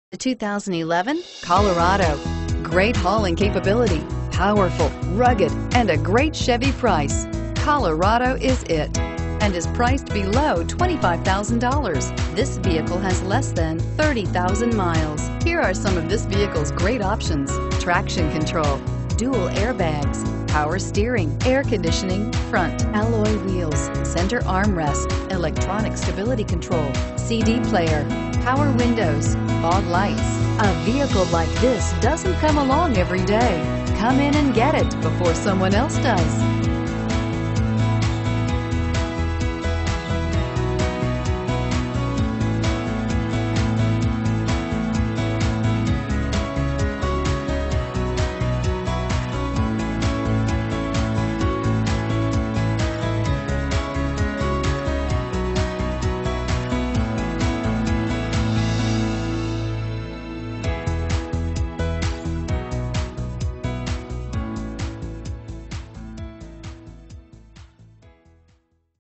2011 Chevrolet Colorado Ankeny IA D14790A
4WD. Red Hot! Flex Fuel! There isn't a nicer 2011 Chevrolet Colorado at this price than this low-mileage creampuff. Don't get stuck in the mudholes of life. 4WD power delivery means you get traction whenever you need it. This Colorado is nicely equipped with features such as 4WD. to view a Free Price Check!